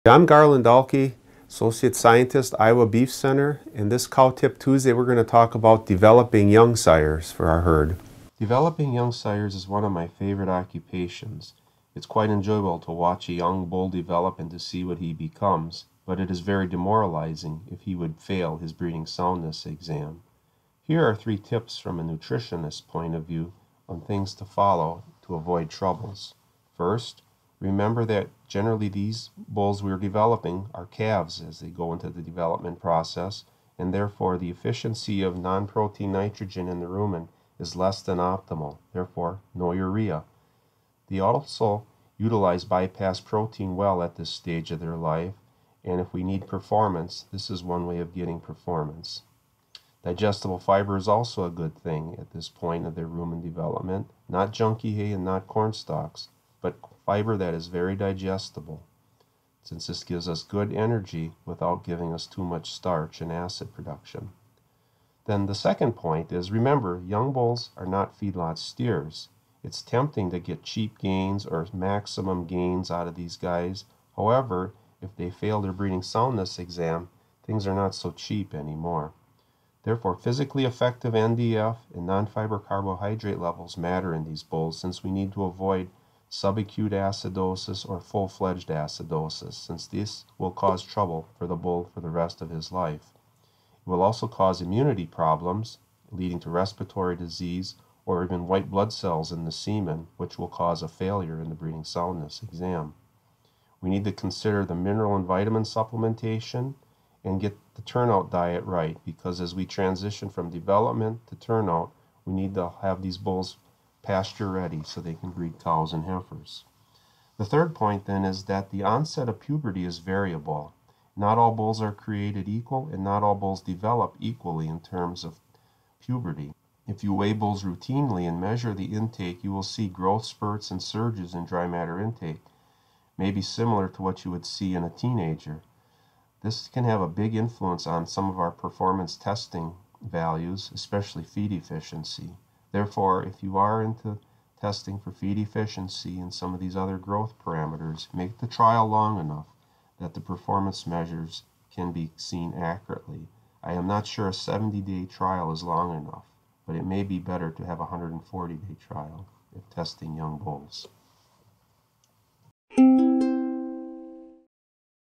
Cow Tip Tuesday Developing Young Sires
This is part of the CowTipTuesday series created by Iowa Beef Center.

Some consider developing bulls a fine art and for good reason. The time between weaning and the first breeding season is critical for young sires, and a misstep here can have significant repercussions. Iowa Beef Center associate scientist Garland Dahlke gives a nutritionist’s perspective for growing young bulls into successful herd sires.